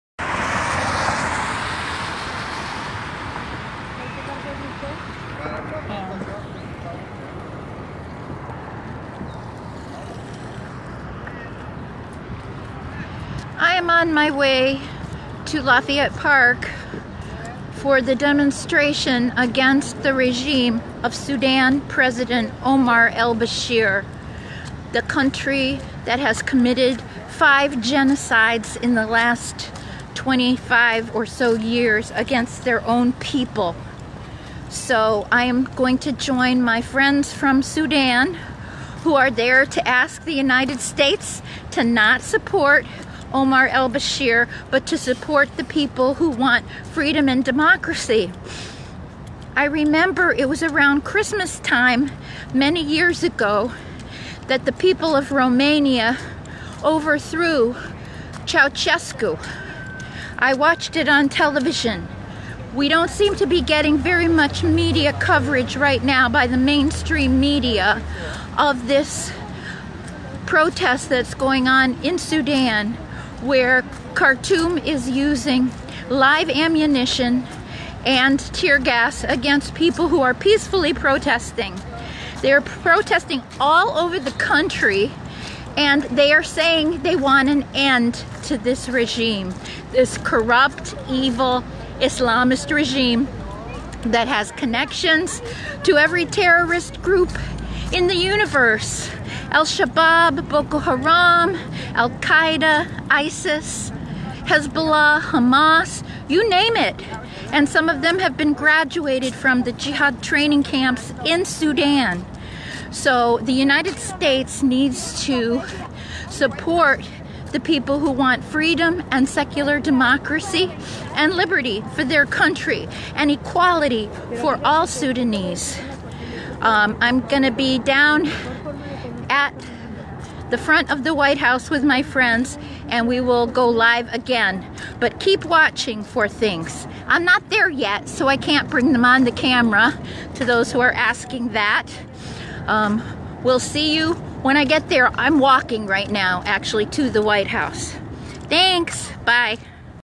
Protesting the evil regime of Omar al Bashir Part One
On Friday, December 28, 2018, Sudanese in America held a rally outside The White House to protest the evil Khartoum regime of Omar al-Bashir in solidarity with the people in Sudan who have been protesting and being met with violent, cruel responses from the regime.